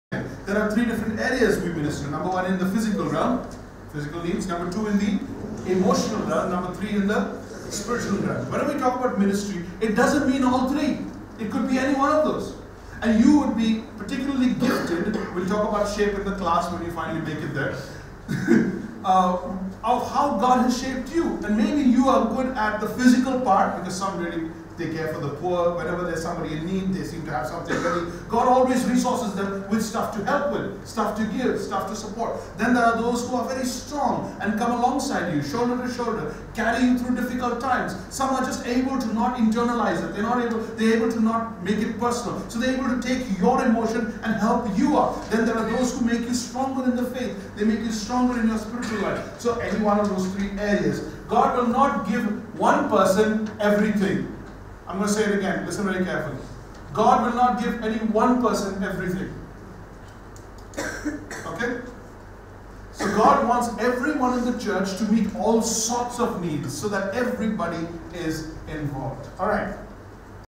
We minister to three area of needs
1. Physical needs
2. Emotional needs
3. Spiritual needs

It could be any one of these.